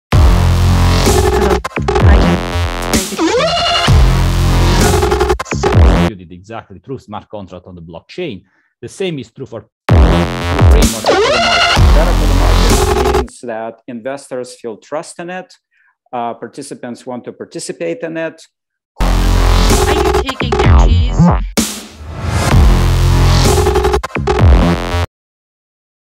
9th Annual Big Data Finance -- recording promo video
The 9th Annual Big Data Finance conference took place on December 9, 2021. Watch this video for a very brief compilation of highlights from the conference. Go to bigdatafinance.org to watch the entire conference (6+ hours of amazing content).